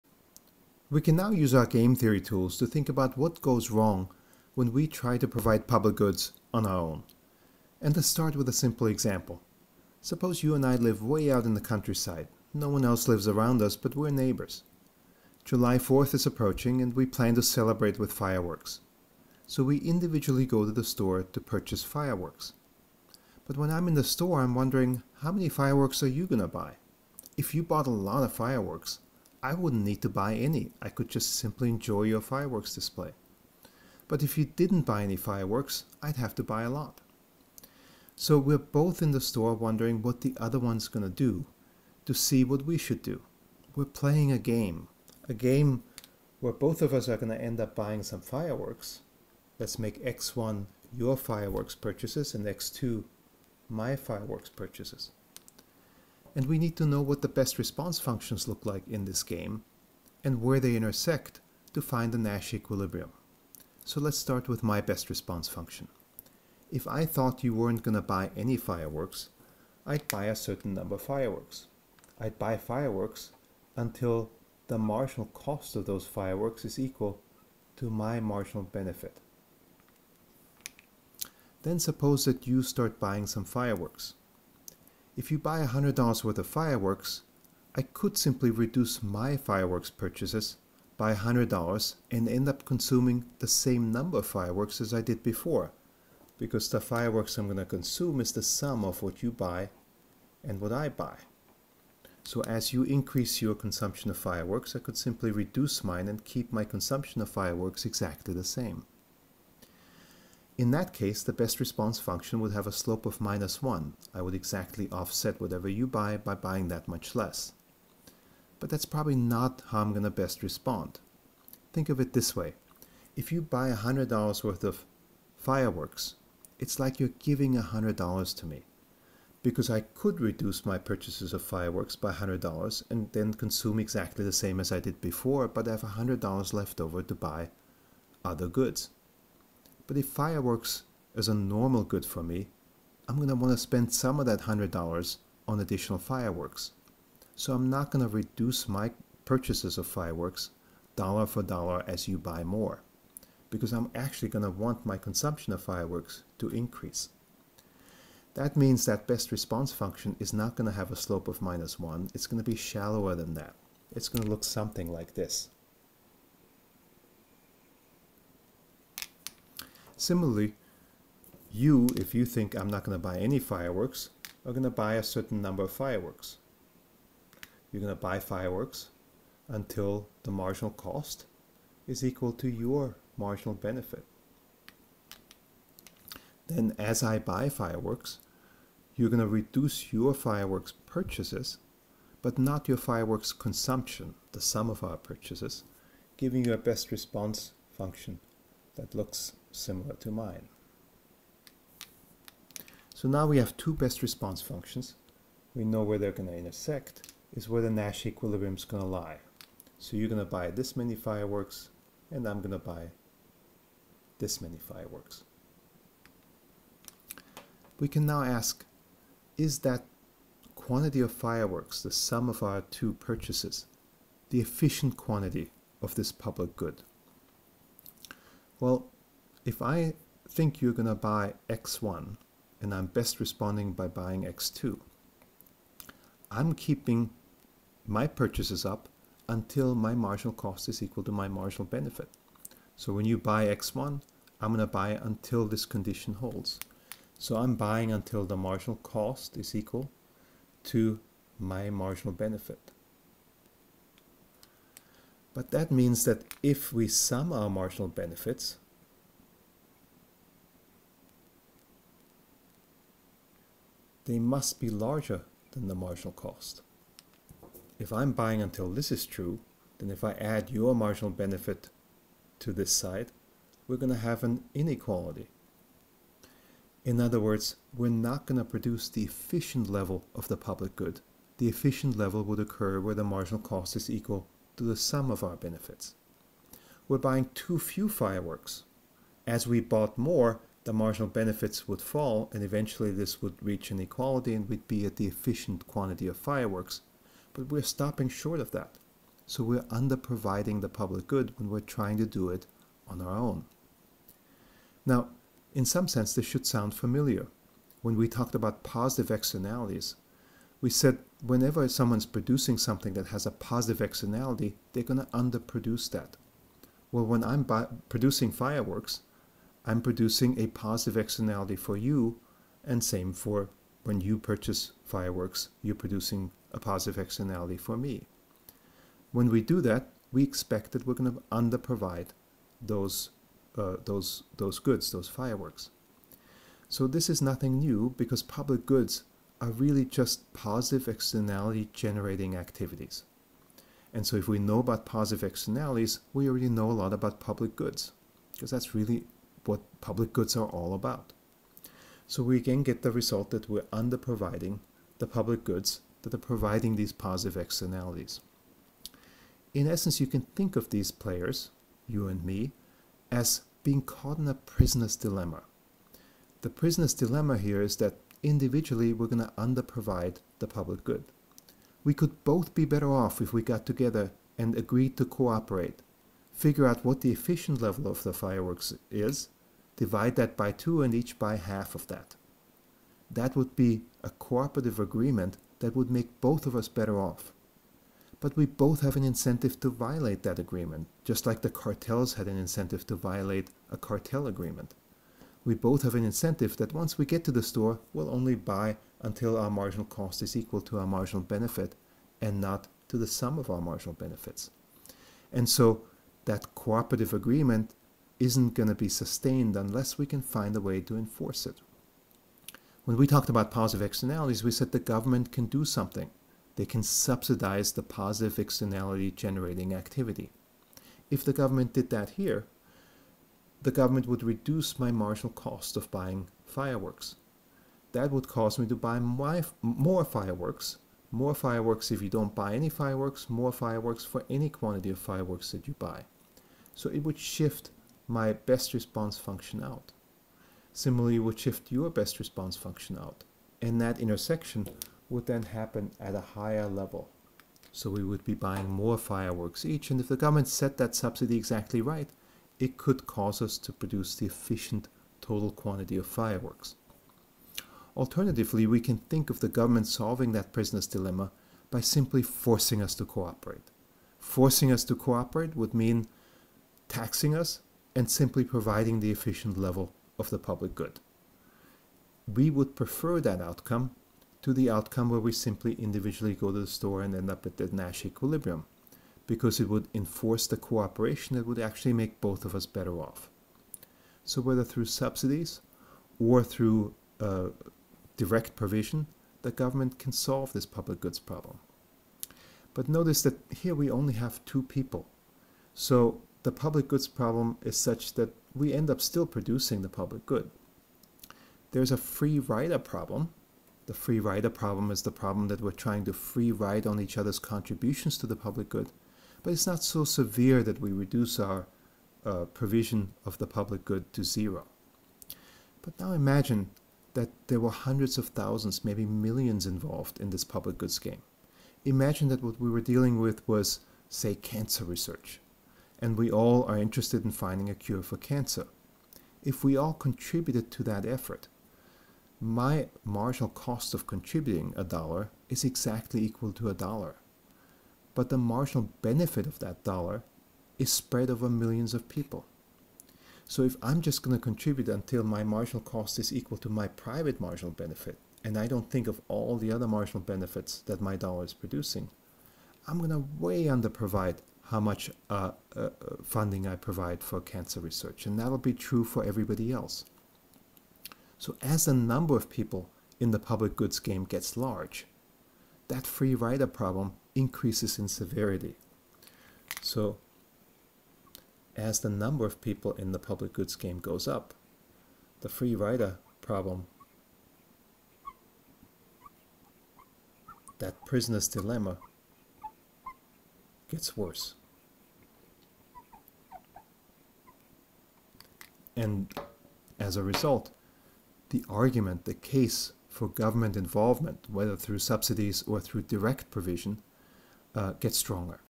29.2.The Free Rider Problem
This video introduces the free rider problem and relates it to private provision of public goods.